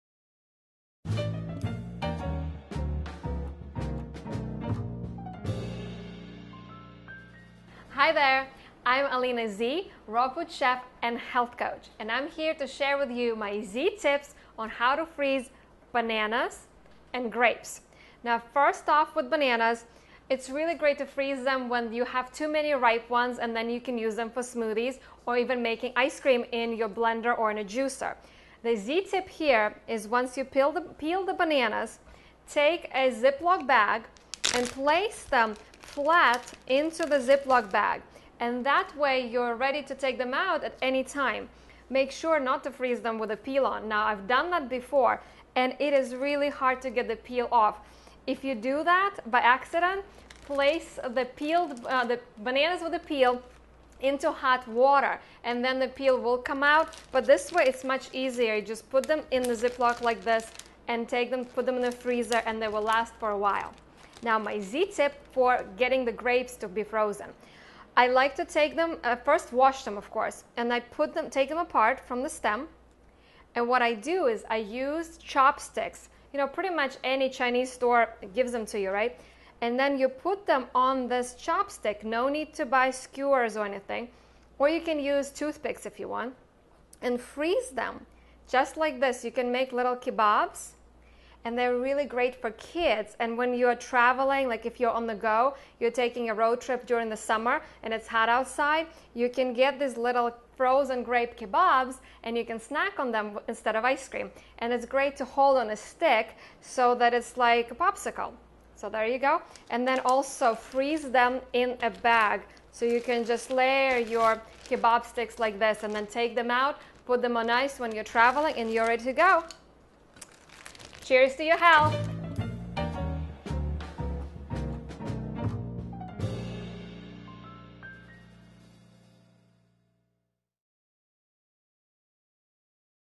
Vegan Diet: How to Freeze Grapes and Bananas
Alina Zhukovskaya, Health Coach and Raw Food Chef shows how to freeze grapes and bananas.